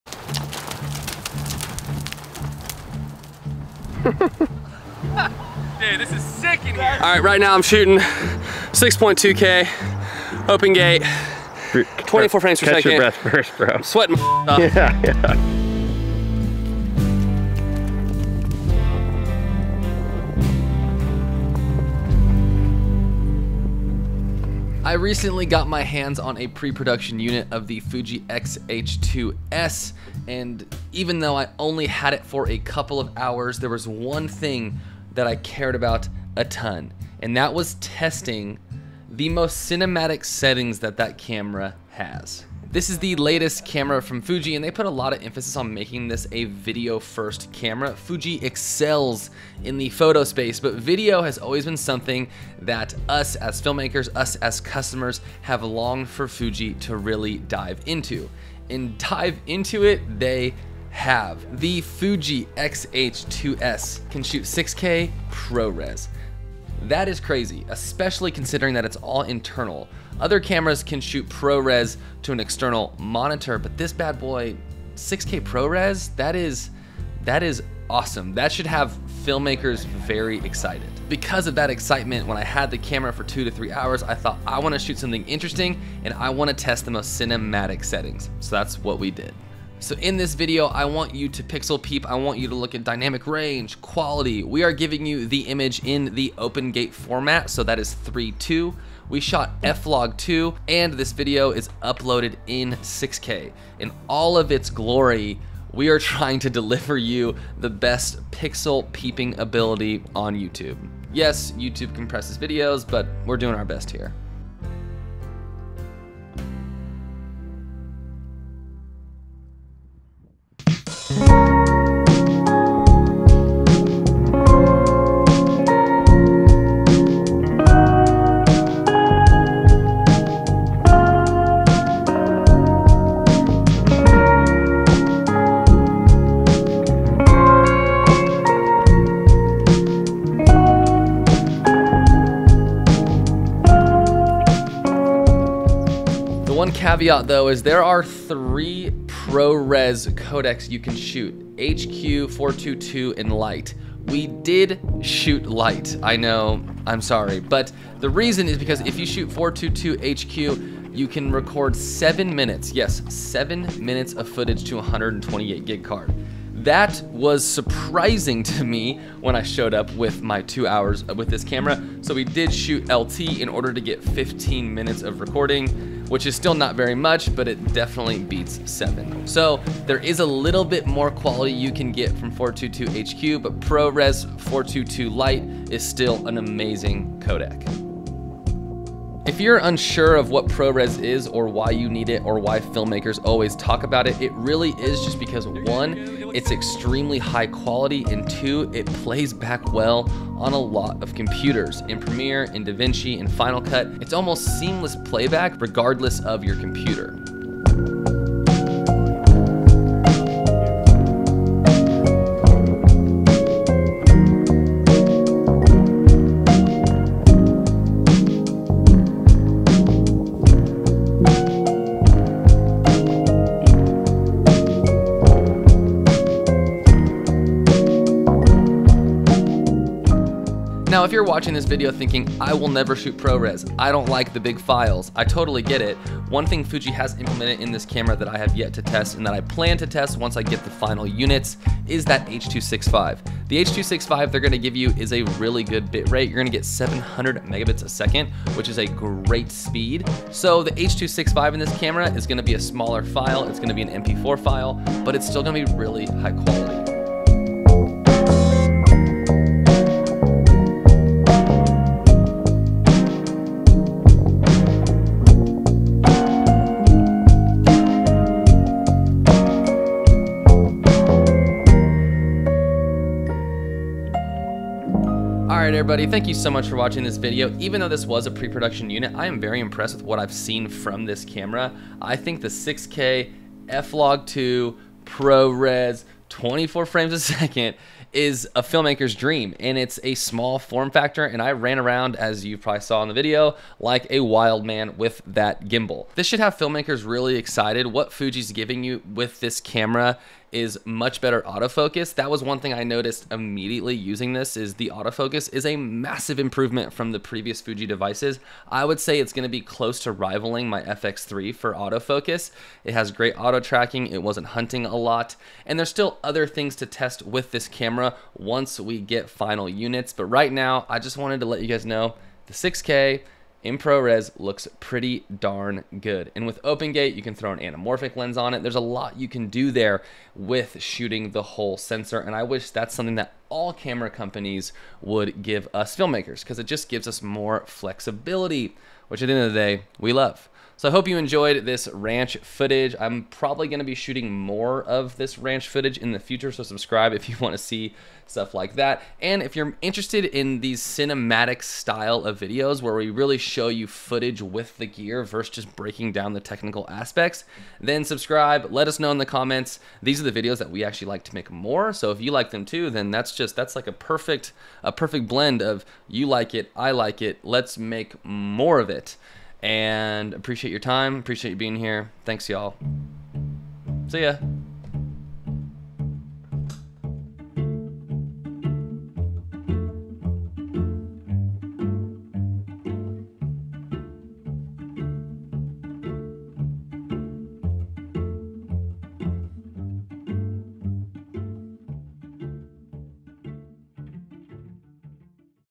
6K Cinematic Test Footage On Fujifilm X-H2s
NOW IN STOCK! X-H2s

The Fujifilm X-H2S is a beast of a camera. More to come!

0:00 - Running Like A Fool
0:14 - First 6K Clips
0:27 - Specs W/ Context
1:58 - 6K Footage
3:11 - What Is Pro Res/More Footage
4:38 - Final Thoughts